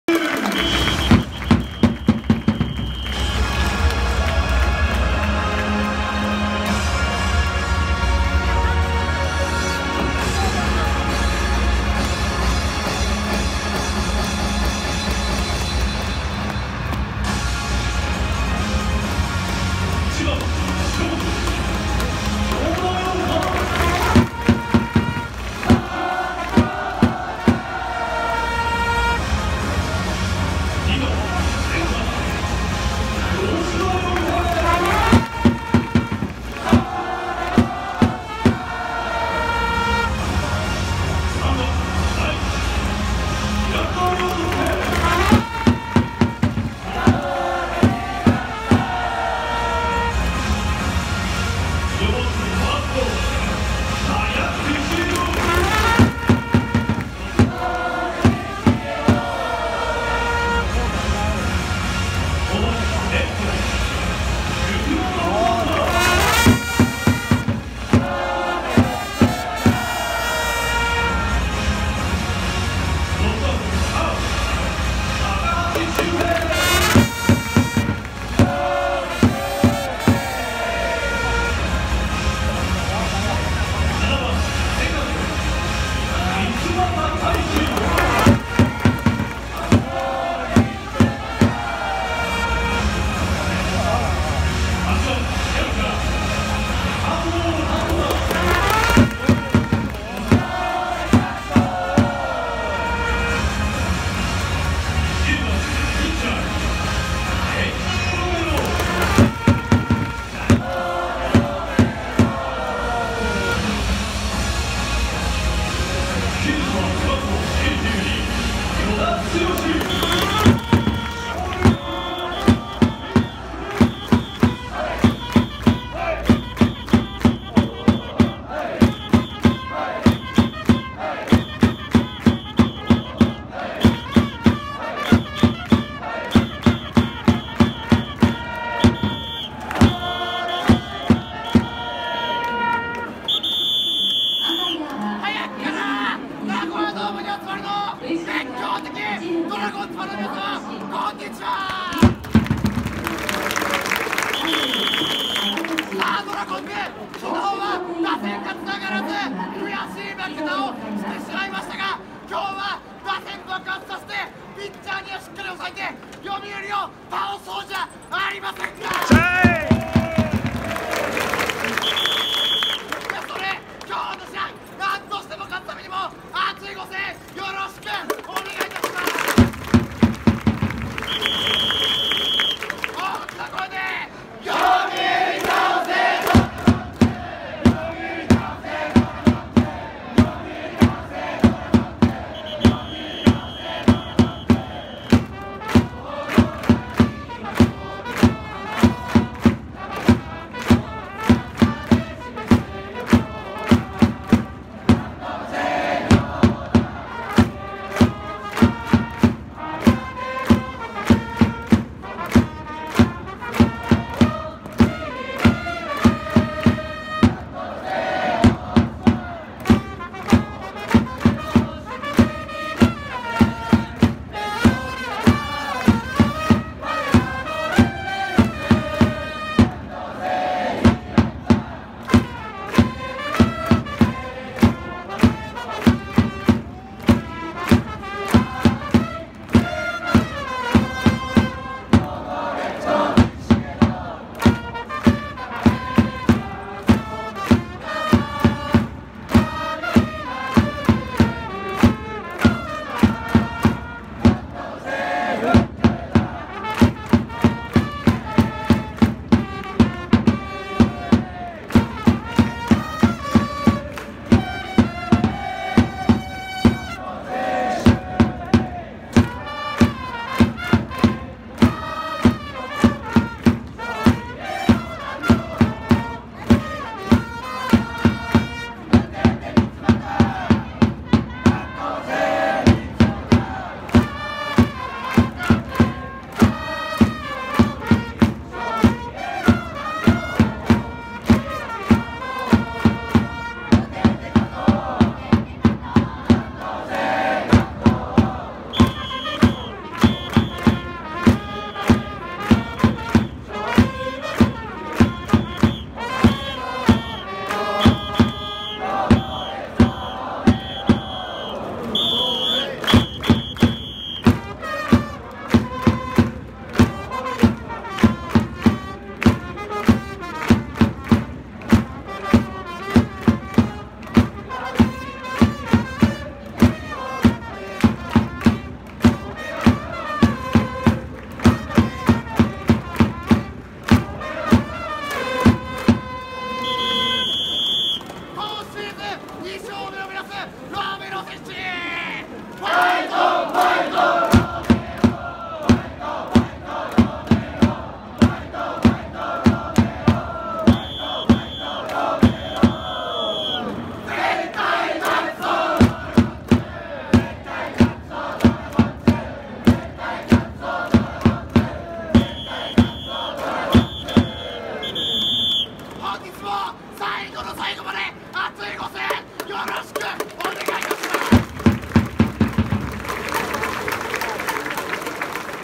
190410 読売対中日 1-9 三ツ俣初スタメン ライスタより(ナゴヤドーム)
ライトスタンドから撮影しました！
三ツ俣選手初スタメンです！